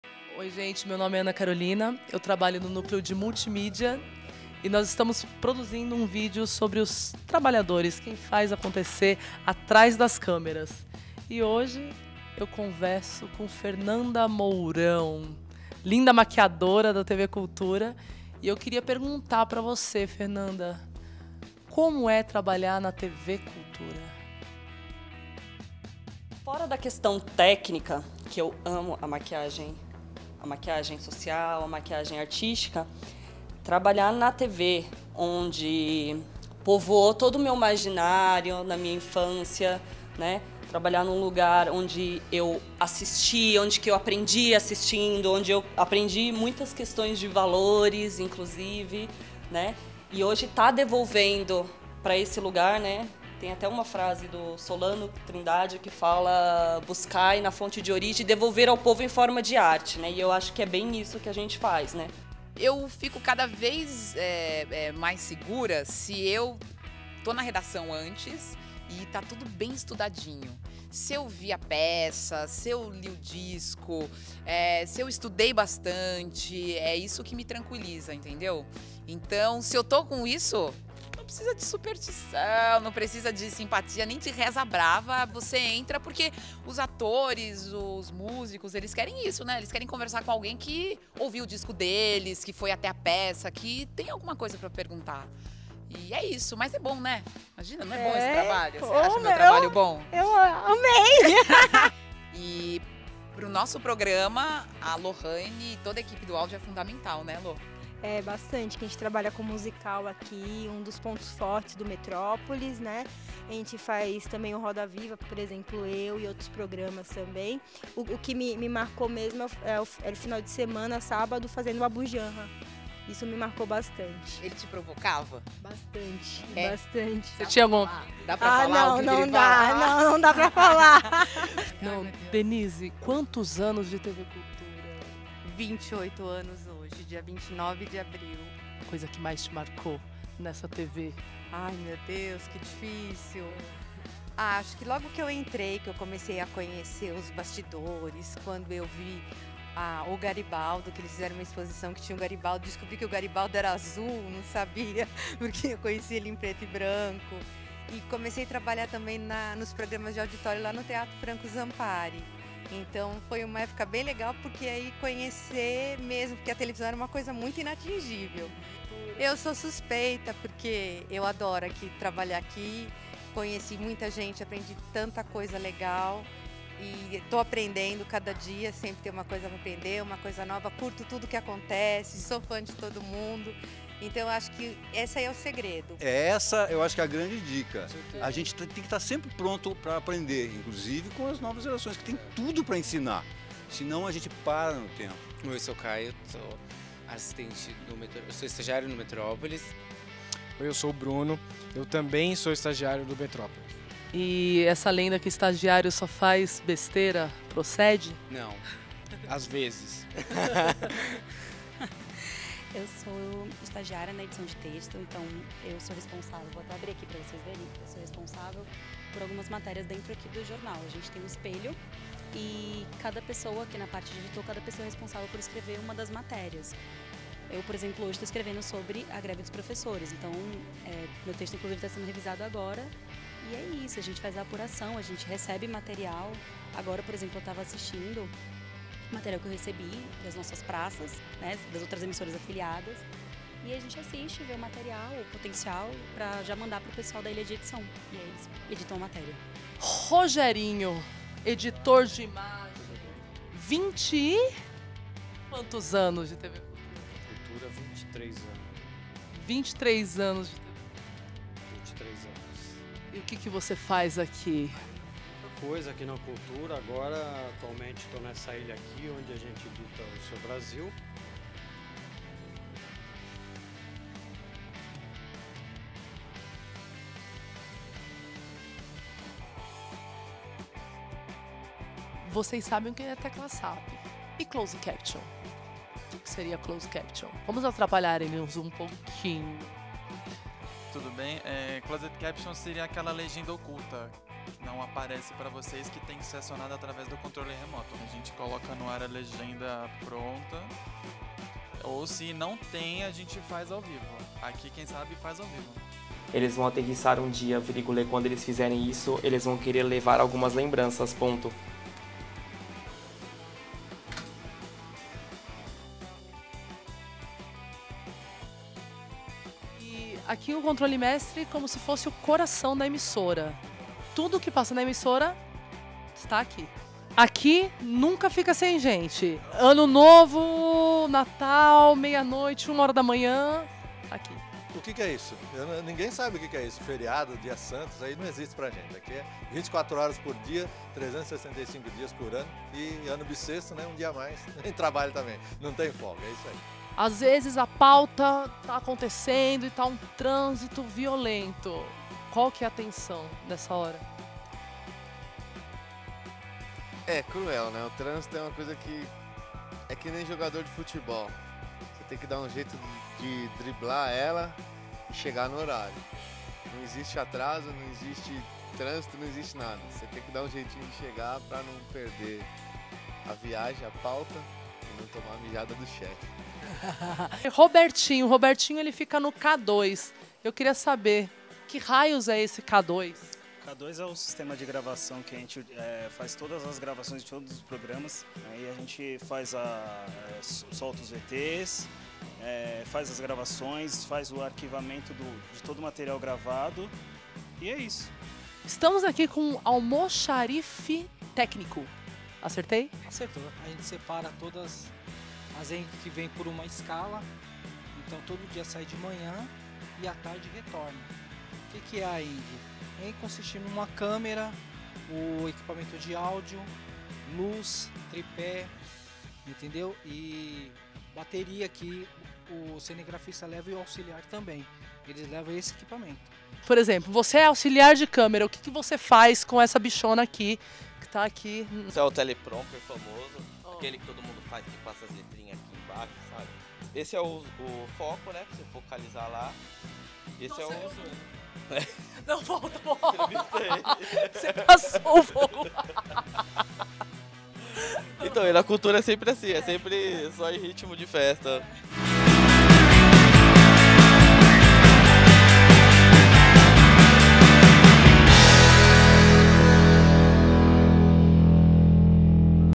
Dia do Trabalho: conheça os bastidores da TV Cultura
Portal cmais+ faz uma homenagem aos funcionários que estão por trás das câmeras